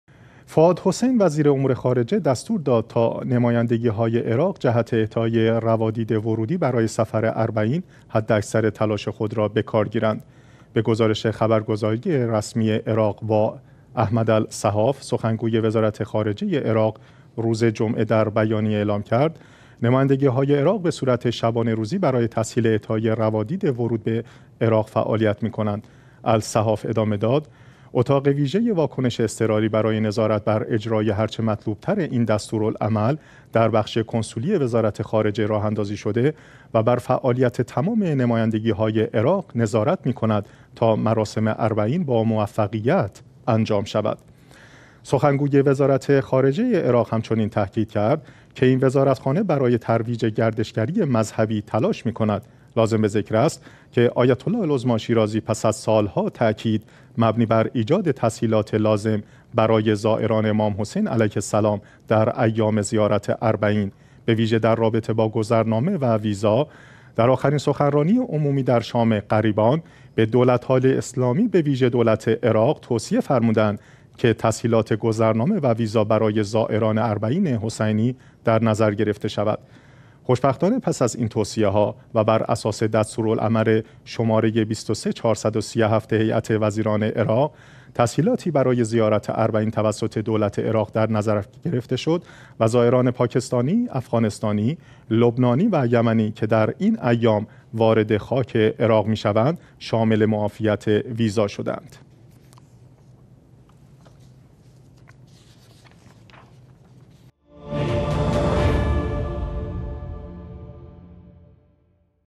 وزیر خارجه عراق درباره صدور روادید در ایام اربعین به سفارتخانه ها دستور ویژه داد
فؤاد حسین وزیر امور خارجه دستور داد تا نمایندگی های عراق جهت اعطای روادید ورودی برای سفر اربعین حداکثر تلاش خود را به کار گیرند.
به گزارش خبرگزاری رسمی عراق (واع) احمد الصحاف، سخنگوی وزارت خارجه عراق روز جمعه در بیانیه ای اعلام کرد: نمایندگی های عراق به صورت شبانه روزی برای تسهیل اعطای روادید ورود به عراق فعالیت می کنند.
الصحاف ادامه داد: اتاق ویژه واکنش اضطراری برای نظارت بر اجرای هر چه مطلوب تر این دستورالعمل ، در بخش کنسولی وزارت خارجه راه اندازی شده و بر فعالیت تمام نمایندگی های عراق نظارت می کند تا مراسم اربعین با موفقیت انجام شود.
سخنگوی وزارت خارجه عراق هم چنین تاکید کرد که این وزارتخانه برای ترویج گردشگری مذهبی تلاش می کند.
لازم به ذکر است که آیت الله العظمی شیرازی پس از سالها تاکید مبنی بر ایجاد تسهیلات لازم برای زائران امام حسین در ایام زیارت اربعین به ویژه در رابطه با گذرنامه و ویزا ، در آخرین سخنرانی عمومی در شام غریبان به دولت های اسلامی به ویژه دولت عراق توصیه فرمودند که تسهیلات گذرنامه و ویزا برای زائران اربعین حسینی در نظر گرفته شود.
خوشبختانه پس از این توصیه ها و بر اساس دستورالعمل شماره ۲۳۴۳۷ هیات وزیران عراق تسهیلاتی برای زیارت اربعین توسط دولت عراق در نظر گرفته شده و زائران پاکستانی، افغانستانی ، لبنانی و یمنی که در این ایام وارد خاک عراق می شوند ، شامل معافیت ویزا شده اند.